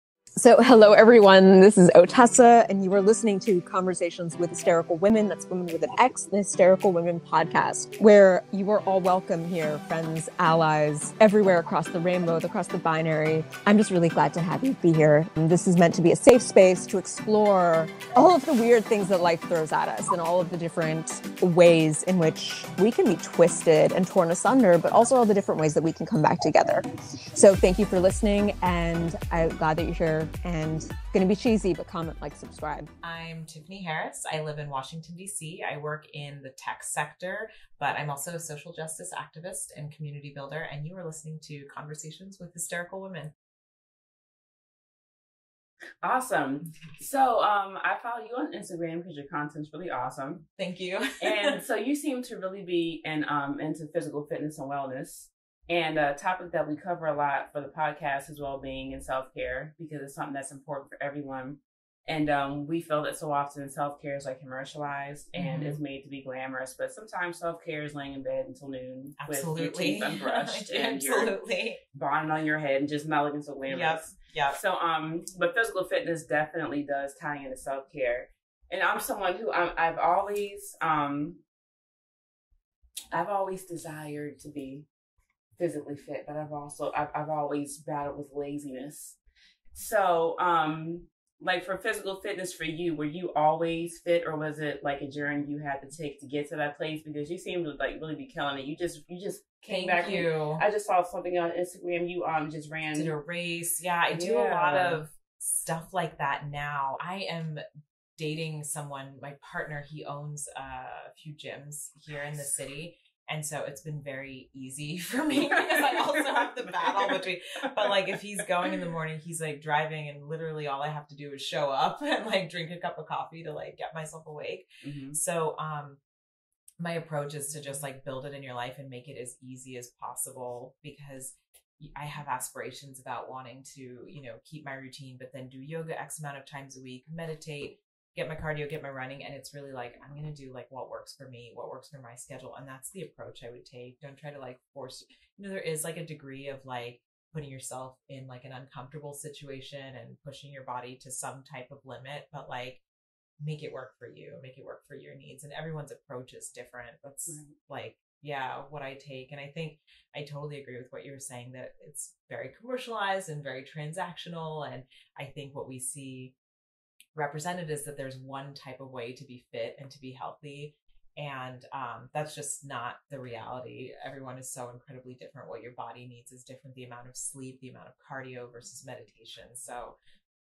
Season 2, Ep. 66 (Teaser): Fitness & Travel
Ft. Tiffany Harris, Chief Program Officer of Moishe House and star of Buzzfeed's Relationshipped.

Hear the full episode on your favorite podcast app!